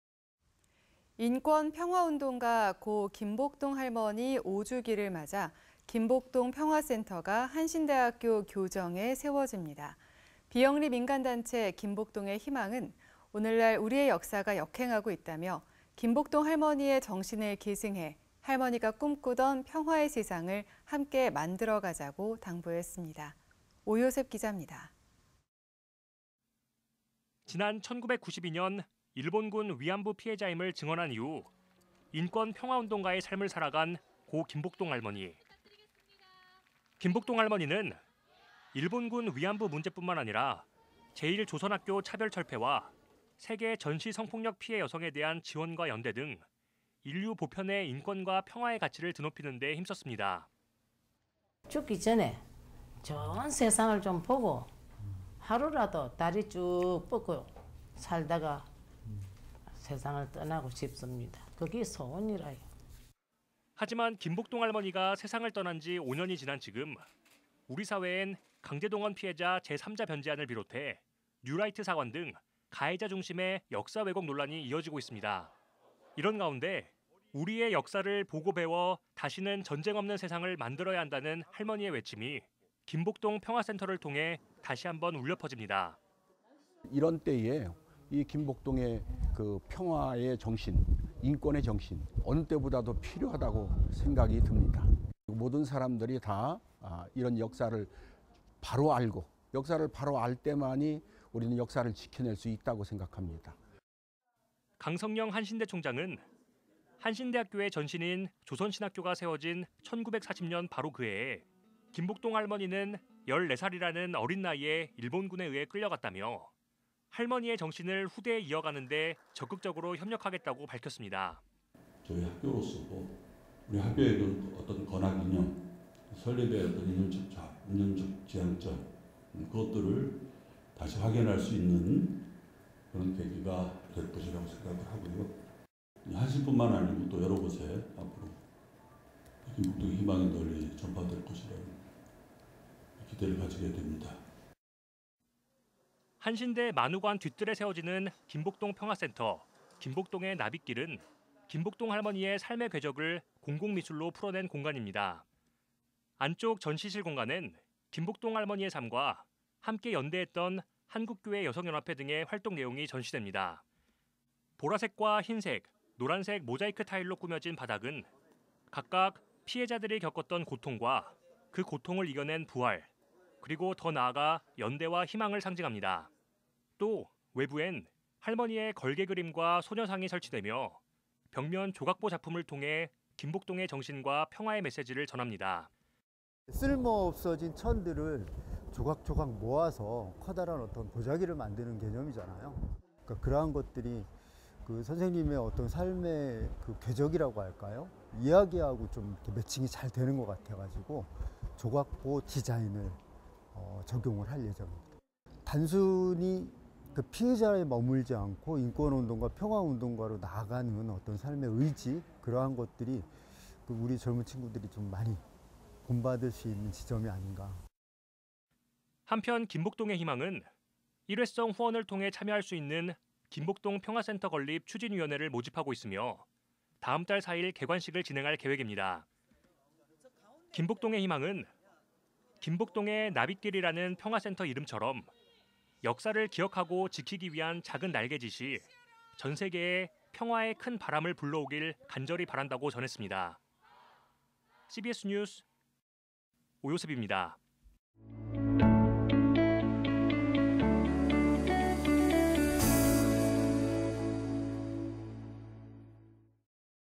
[CBS 뉴스] '인권과 평화의 가치' 전하는 김복동 평화센터 건립
[앵커]
인권평화운동가 고 김복동 할머니 5주기를 맞아 김복동 평화센터가 한신대학교 교정에 세워집니다.

비영리민간단체 김복동의 희망은 "오늘날 우리의 역사가 역행하고 있다"며 "김복동 할머니의 정신을 계승해 할머니가 꿈 꾸던 평화의 세상을 함께 만들어가자"고 당부했습니다.

오요셉 기자입니다.

[기자]
지난 1992년 일본군'위안부' 피해자임을 증언한 이후 인권평화운동가의 삶을 살아간 고 김복동 할머니.

김복동 할머니는 일본군 '위안부' 문제뿐만 아니라, 재일조선학교 차별 철폐와 세계 전시 성폭력 피해 여성에 대한 지원과 연대 등 인류 보편의 인권과 평화의 가치를 드높이는 데 힘썼습니다.

[김복동 할머니 / 2016년 2월 2일]
"죽기 전에 좋은 세상을 보고, 하루라도 다리 쭉 뻗고 살다가 세상을 떠나고 싶습니다. 그게 소원이라요."

하지만 김복동 할머니가 세상을 떠난 지 5년이 지난 지금, 우리 사회엔 강제동원 피해자 '제3자 변제안'을 비롯해 뉴라이트 사관 등 가해자 중심의 역사 왜곡 논란이 이어지고 있습니다.

이런 가운데, '우리의 역사를 보고 배워 다시는 전쟁 없는 세상을 만들어야 한다'는 할머니의 외침이 '김복동 평화센터'를 통해 다시 한 번 울려 퍼집니다.

비영리민간단체 김복동의 희망은 "우리는 역사가 역행하고, 인권과 평화가 위협 받는 오늘을 살고 있다"며 "김복동의 삶과 정신을 기억하고 계승해 김복동이 바라고 살았던 세상을 만들어가자"고 평화센터 설립 취지를 밝혔습니다.

[박상필 목사 / 김복동평화센터 추진위원회 상임위원장]
"김복동의 평화의 정신, 인권의 정신이 그 어느 때보다 필요하다는 생각이 듭니다. (김복동 할머니의) 이런 활동은 개인의 활동이 아니라 나가서 모든 후배들이 기억하고 동시대인들이 함께 해야할 일입니다. 모든 사람들이 이런 역사를 바로 알고, 역사를 바로 알 때만이 우리는 역사를 지켜낼 수 있다고 생각합니다."

강성영 한신대 총장은 "한신대학교의 전신인 조선신학교가 세워진 1940년, 바로 그 해에 김복동 할머니는 14살이라는 어린 나이에 일본군에 의해 끌려갔다"며 "할머니의 정신을 후대에 이어가는데 적극적으로 협력하겠다"고 밝혔습니다.

[강성영 총장 / 한신대학교]
"저희 학교로서도 학교의 건학 이념, 설립과 운영 지향점, 그것들을 다시 확인할 수 있는 계기가 될 것이라고 생각하고요. 우리 한신뿐만 아니라 여러 곳에 앞으로 김복동의 희망이 널리 전파될 것이라는 기대를 가지게 됩니다."

한신대 만우관에 뒷 뜰에 세워지는 김복동 평화센터, '김복동의 나비 길'은 김복동 할머니의 삶의 궤적을 공공미술로 풀어낸 공간입니다.

안쪽 전시실 공간엔 김복동 할머니의 삶과 함께 연대했던 한국교회여성연합회 등의 활동 내용 등이 전시됩니다.

보라색과 흰색, 노란색 모자이크 타일로 꾸며진 바닥은 각각 피해자들이 겪었던 고통과, 그 고통을 이겨낸 부활, 그리고 더 나아가 연대와 희망을 상징합니다.

또, 외부엔 할머니의 걸개 그림과 소녀상이 설치되며 벽면 조각보 작품을 통해 김복동의 정신과 평화의 메세지를 전합니다.

[이구영 작가 ]
"(조각보란) 쓸모 없어진 천들을 조각조각 모아서 커다란 보자기를 만드는 개념이잖아요. 그런 것들이 선생님의 삶의 이야기와 잘 부합되는 것 같아서 조각보 디자인을 적용할 예정입니다. 단순히 피해자에 머물지 않고 인권운동가, 평화운동가로 나아가는 삶의 의지, 그런 것들이 우리 젊은 친구들이 많이 본받을 수 있는 지점이 아닌가 (생각합니다.)"

한편, 김복동의 희망은 일회성 후원을 통해 참여할 수 있는 김복동 평화센터 건립 추진위원회를 모집하고 있으며, 다음달 4일 개관식을 진행할 계획입니다.

김복동의희망은 "'김복동의 나비 길'이라는 평화센터 이름처럼 역사를 기억하고 지키기 위한 작은 날개 짓이 전세계에 평화의 큰 바람을 불러오길 간절히 바란다"고 전했습니다.